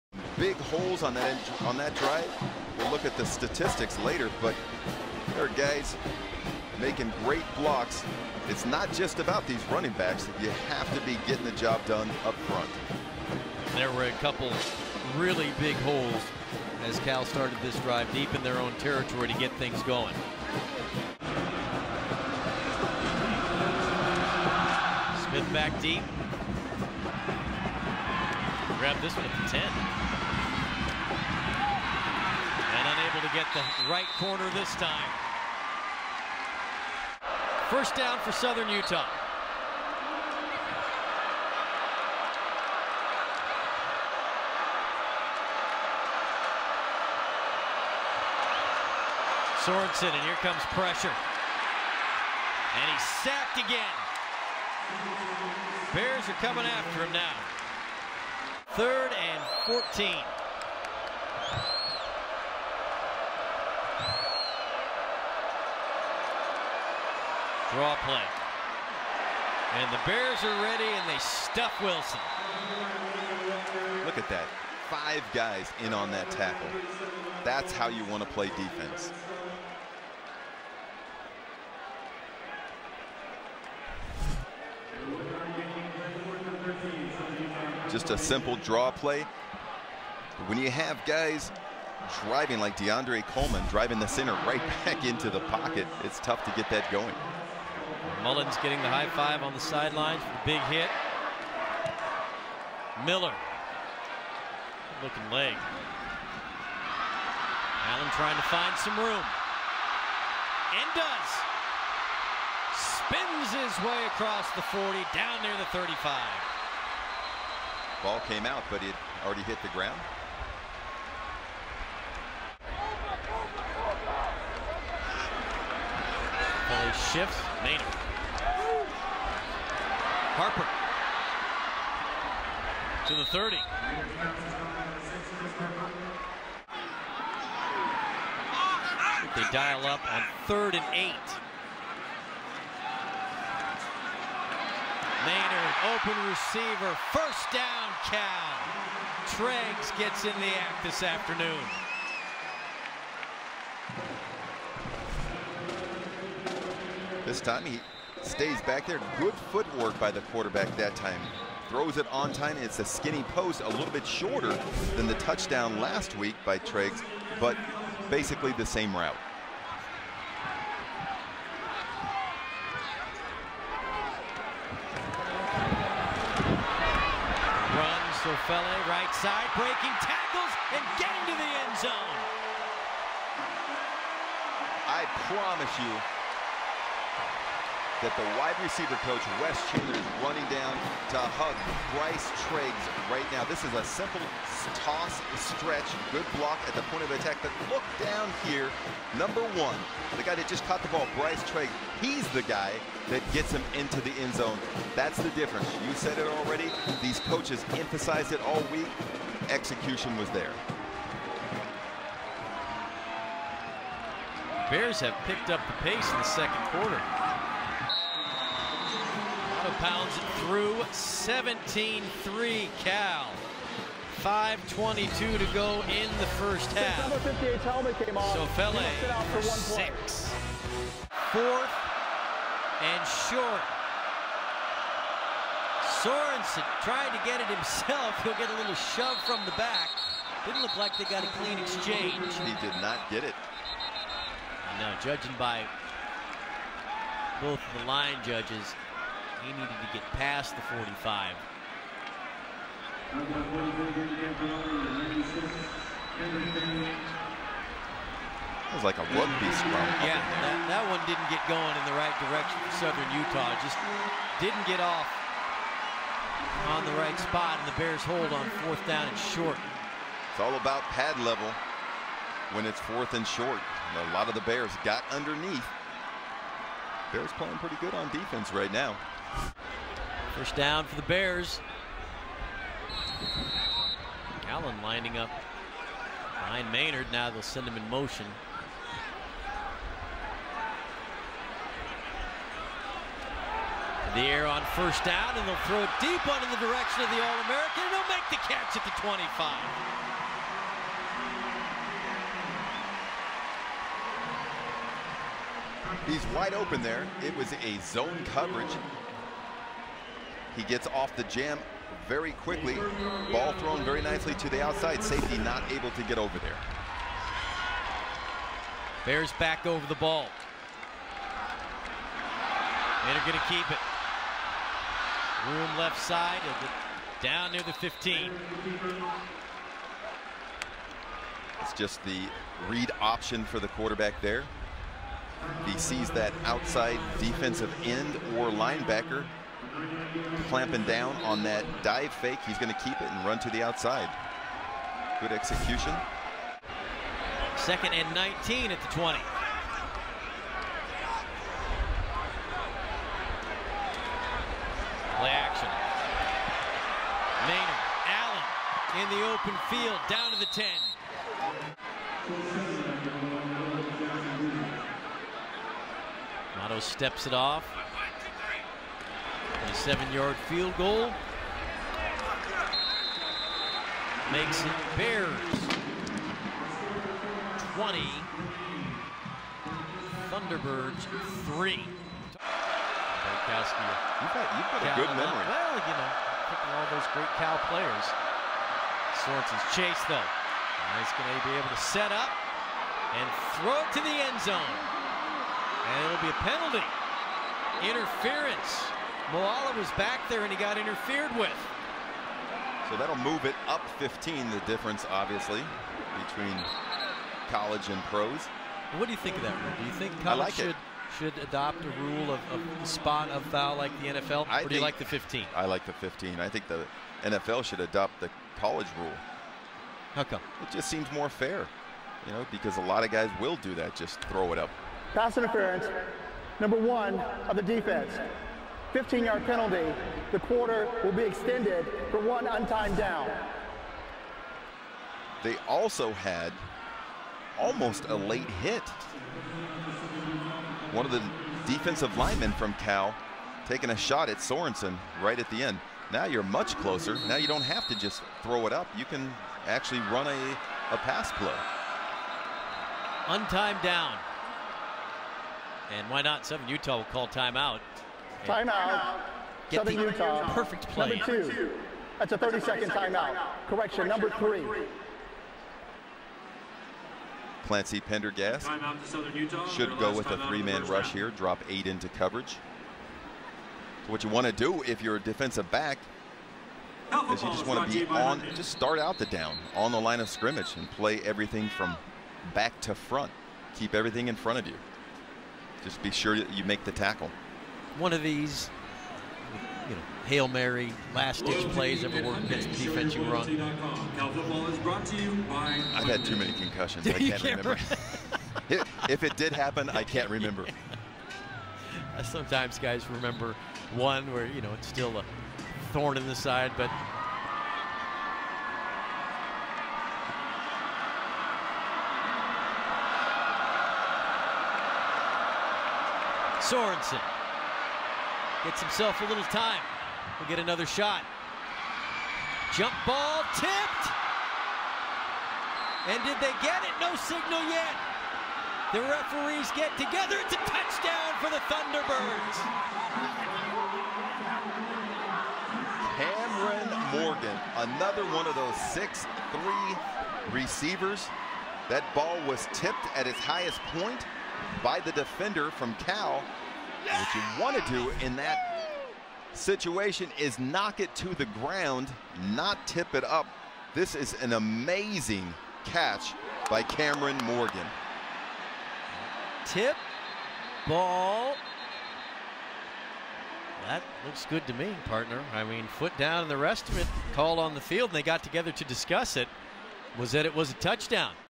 Cal Football Highlights - Southern Utah 2012 - Part 2
Cal Highlights only. Thanks to TouchedTheAxeIn82 who made this video possible.